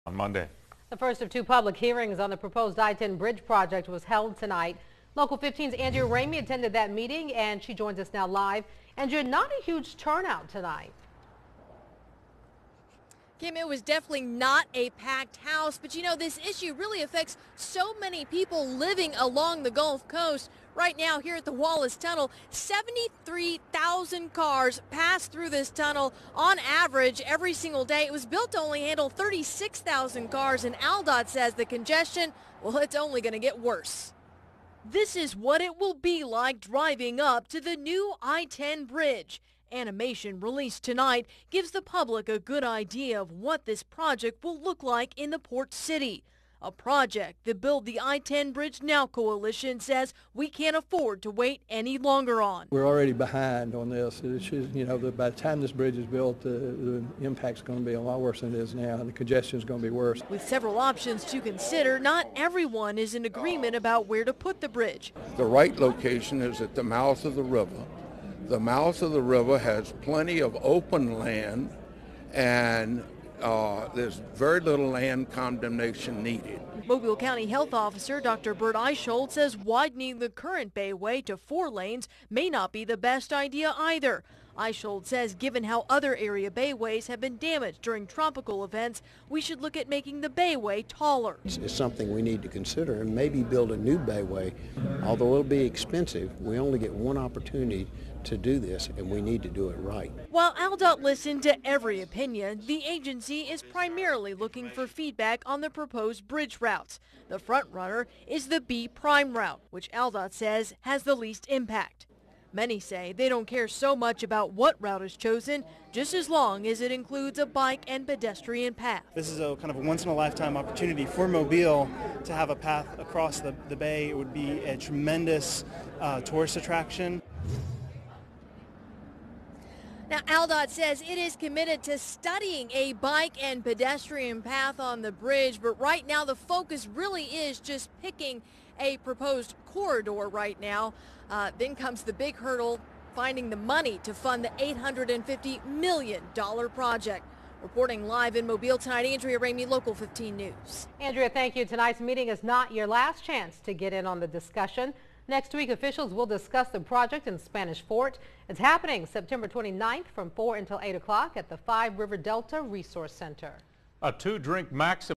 Public Hearings on 1-10 Bridge Proposal
MOBILE, Ala. (WPMI) The first of two public hearings on the proposed I-10 bridge project was held Tuesday night. Thirty-one people signed up to speak, including Mike Lee with the Build the 1-10 Bridge Now Coalition. Lee says the community can't wait any longer for the bridge.

"We're already behind on this. By the time this bridge is built, the impact is going to be a lot worse than now. The congestion is going to be worse," said Lee.

Right now, 73,000 cars pass through the Wallace Tunnel on an average day. It was built to handle 36,000 daily, and ALDOT says the congestion there will only get worse. The project also calls for widening the Bayway to four lanes.

Mobile County Health Officer Dr. Bert Eichold says that may not be the best idea. Eichold says given how other area bridges have been damaged during tropical events, we should look at making the Bayway taller.

"It's something we need to consider and maybe build a new Bayway. Although it'll be expensive, we only get one opportunity to do this and we need to do it right," said Eichold.

While ALDOT listened to every opinion, the agency is primarily looking for feedback on the proposed bridge routes. The front runner is the B prime route, which ALDOT says it has the least impact.

Many say they don't care so much about what route is chosen, just as long as it includes a bike and pedestrian path.

"This is a once in a lifetime opportunity for Mobile to have a path across the bay. It would be a tremendous tourist attraction," said Ben Brenner.

ALDOT says it is committed to studying a bike and pedestrian path on the bridge, but right now the focus is on selecting the route. Then comes the big hurdle of how to fund the $850 million project.

The next public hearing is on September 29 from 4 until 8 p.m. at the Five Rivers Delta Resource Center.